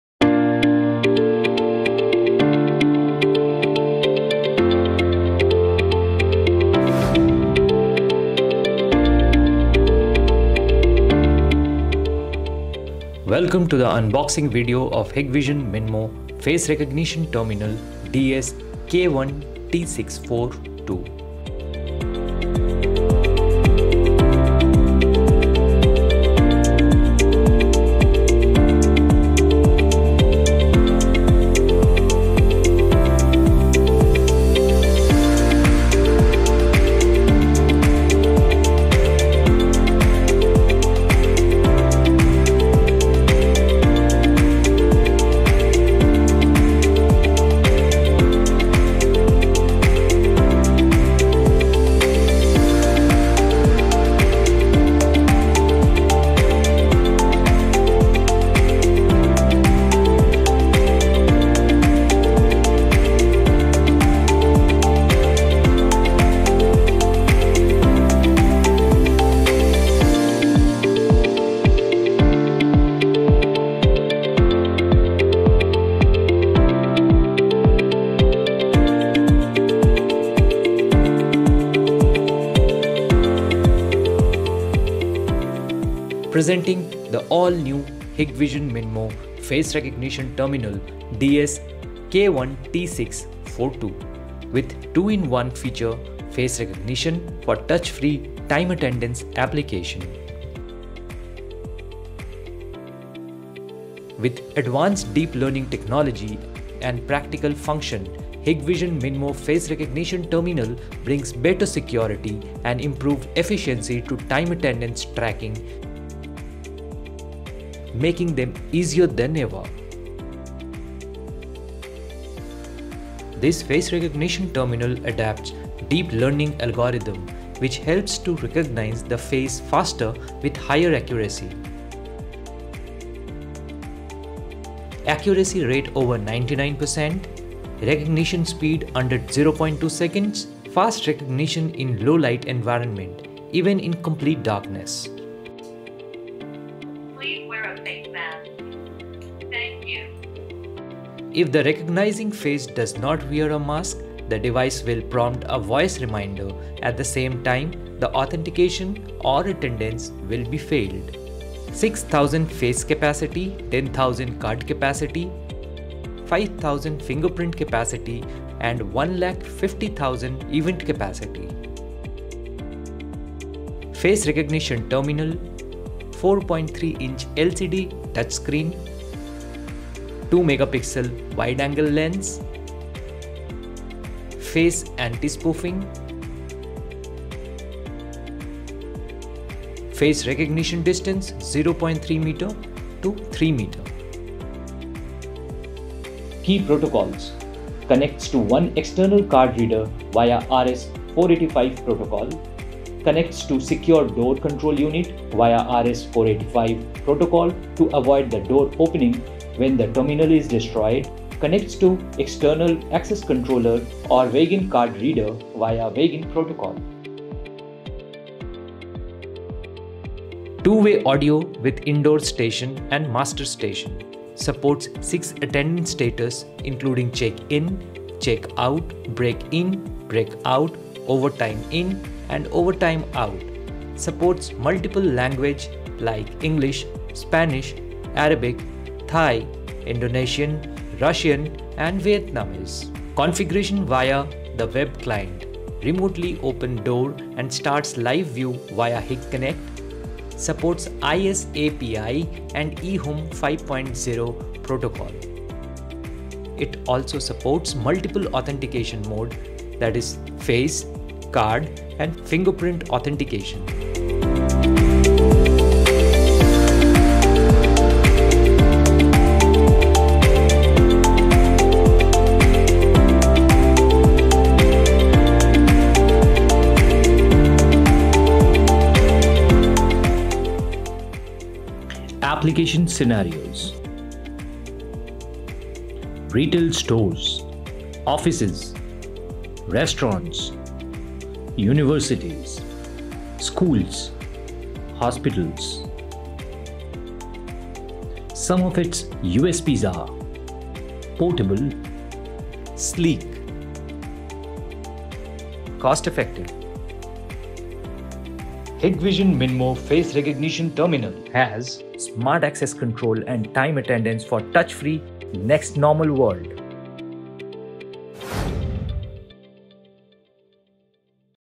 Hikvision Face recognition Terminal DS-K1T642 - MinMoe Series
Presenting the Hikvision Face Recognition Terminal DS-K1T642 in the access control segment, the MinMoe series. This is an addition to our Unboxing series. In this video, we showcase the latest Minmoe product, its USPs, Application Scenarios, and its various key features like advanced deep learning technology, practical functions, touch-free, face anti-spoofing, and many more.